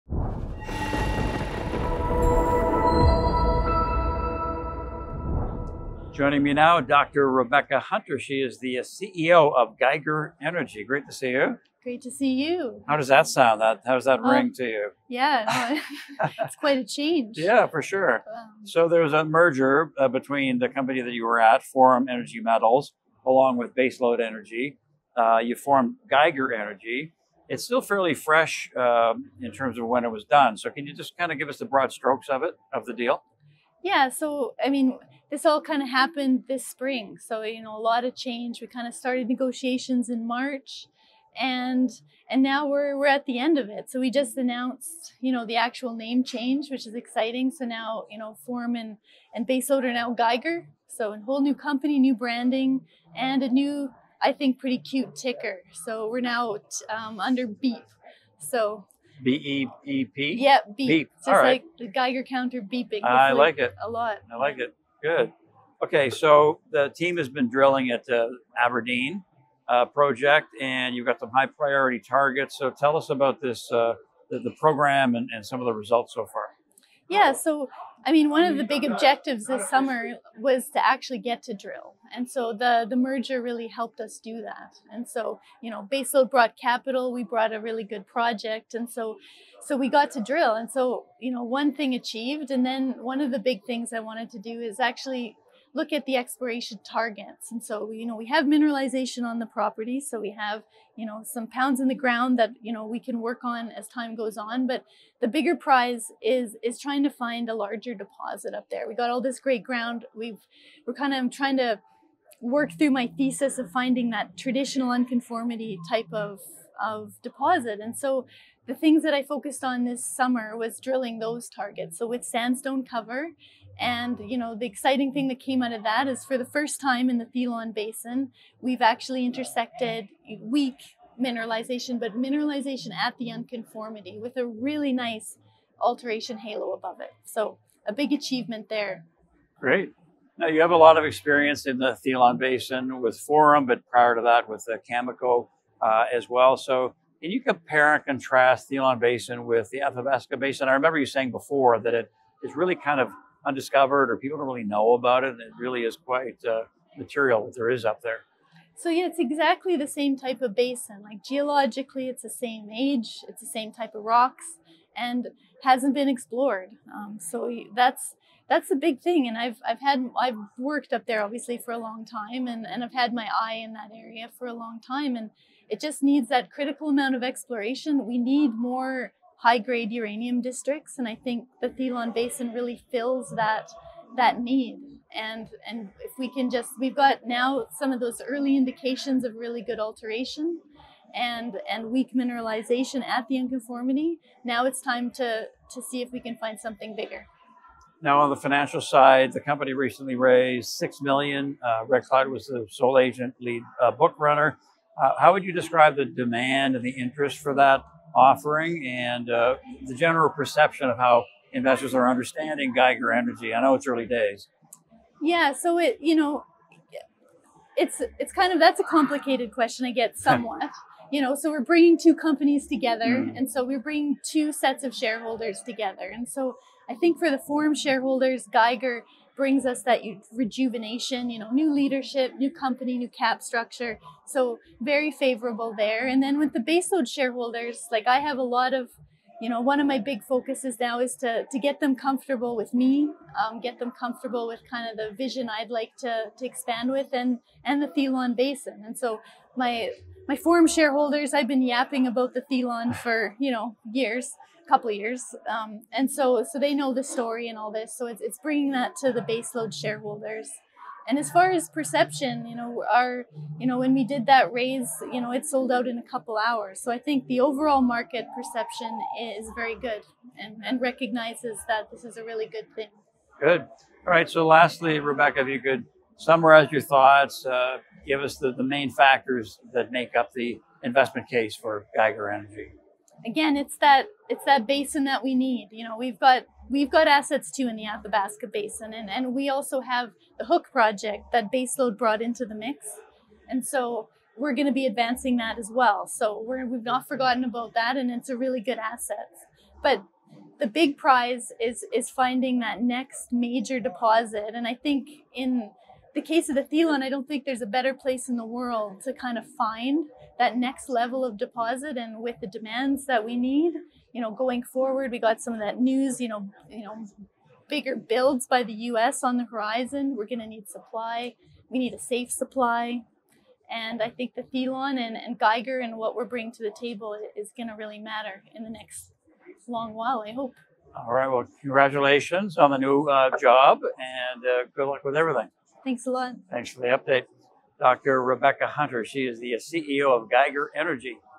Geiger Energy: Mergers, Aberdeen Drilling & More! | Red Cloud’s Fall Mining Showcase 2025
Dr. Rebecca Hunter, CEO of Geiger Energy, joined us at Red Cloud’s Fall Mining Showcase in Toronto to provide an update on the recent merger of Forum Energy Metals and Baselode Energy to form Geiger Energy. She discussed the Aberdeen drilling program and key high-priority targets, compared opportunities in the Thelon Basin to the Athabasca Basin, and shared insights on the $6 million financing led by Red Cloud, including market demand and perception. Hunter also summarized the investment case for Geiger amid a strong uranium sector backdrop.